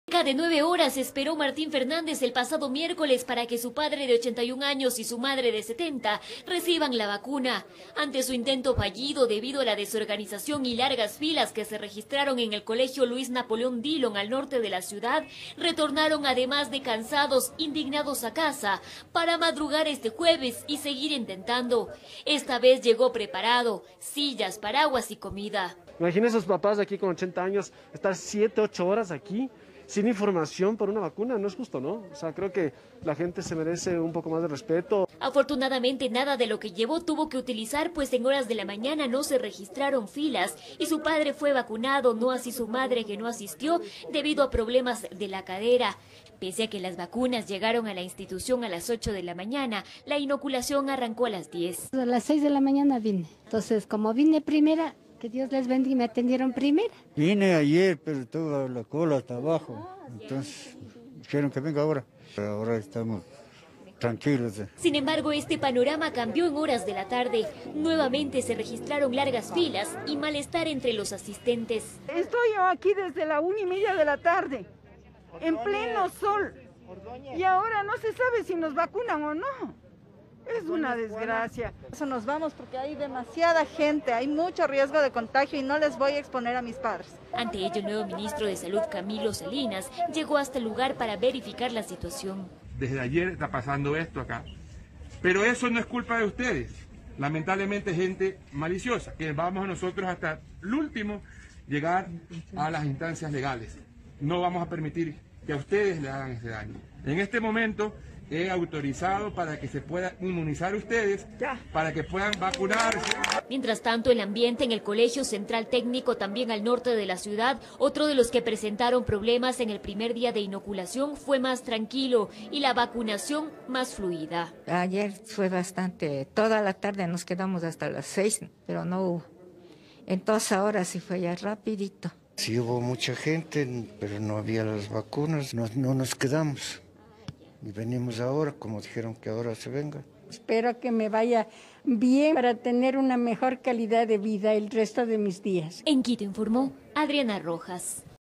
Continúan largas filas para vacunarse contra Covid 19 en colegio Dillon
Hasta nueve horas han tenido que esperar para inmunizarse

En Quito se registraron, nuevamente, largas filas y desorganización durante la vacunación contra Covid 19 en los exteriores del Colegio Luis Napoleon Dillon, mientras que en el Central Técnico el proceso fue más fluido. El ministro de Salud, Camilo Salinas, acudió a estos puntos para vigilar la aplicación de dosis.